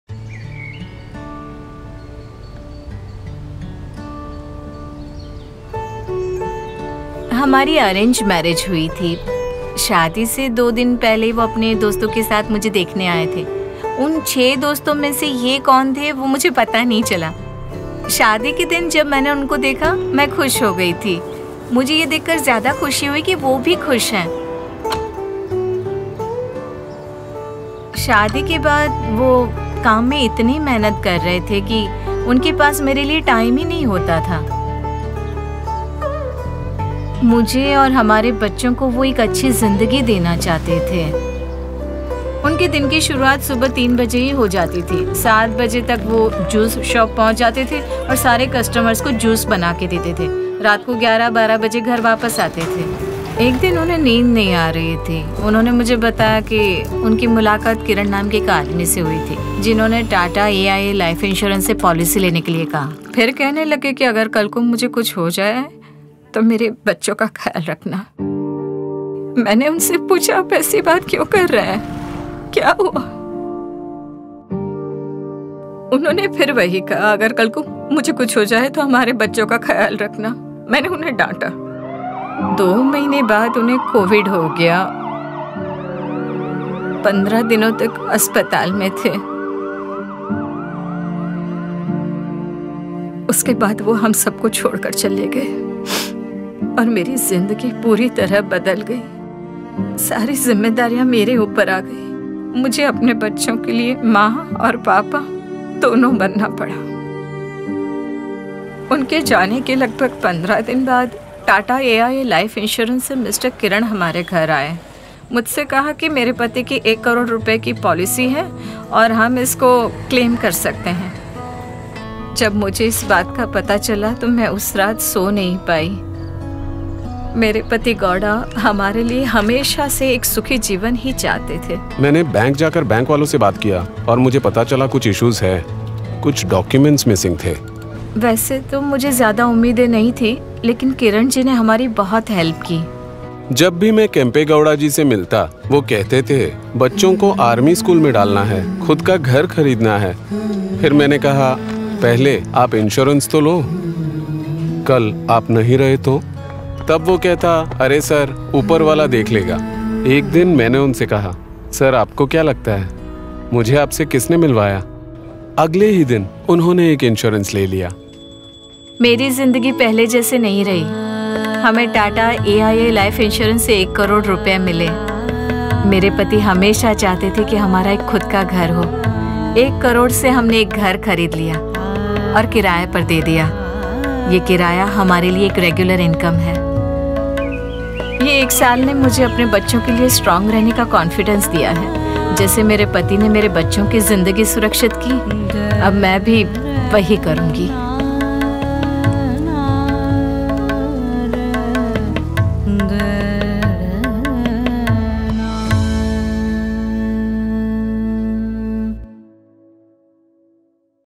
‘The Legacy of Life’ – The Inspiring Story of Kempe & Manuja
Life is unpredictable but your plans don’t have to be! Don't miss this inspiring tale that highlights how life insurance can be your lifeline of dreams and aspirations, helping you every challenge you may face….

Join us as we salute the indomitable spirit of Manuja Gowda, who stood up to the challenges of life, with some help from her husband Kempe, and his legacy of love!